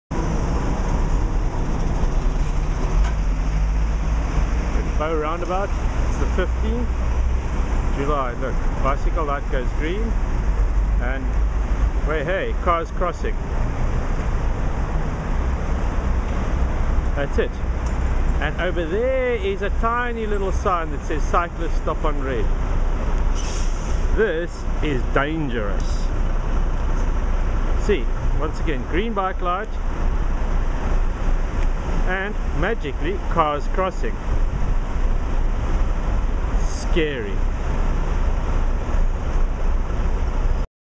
Cyclists killed on the Dangerous and Deadly Bow Roundabout
The Bow Roundabout is becoming famous for killing cyclists.
I think I have finally found out why.
The green bicycle shows clearly in favour of the cyclist but the cross traffic is allowed to cross the cyclists path simultaneously on a green light.